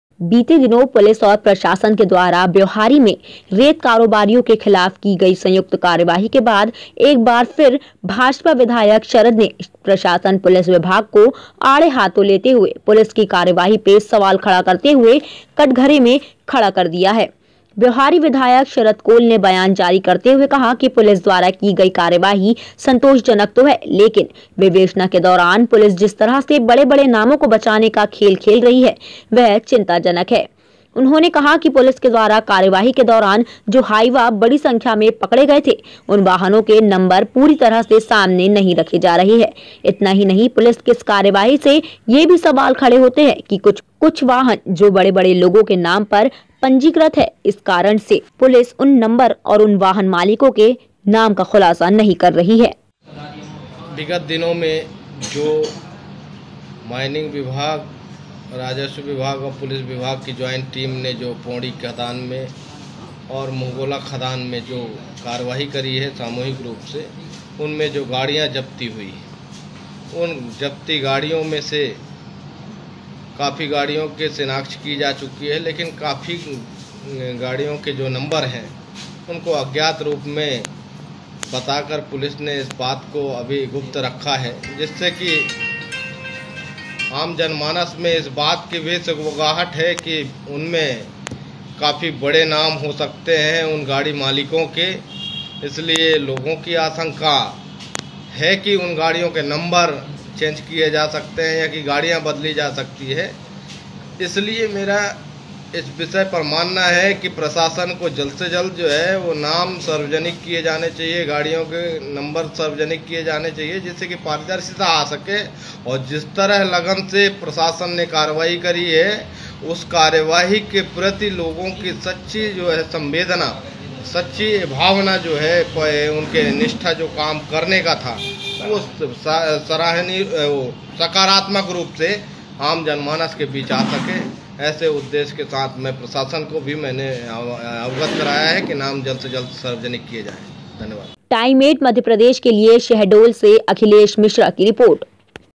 पुलिस कार्यवाई पर विधायक को एतराज। TIME8 MADHYA PRADESH
शहडोल। रेत के खिलाफ पुलिस की कार्यवाही पर विधायक ने खड़े किए सवाल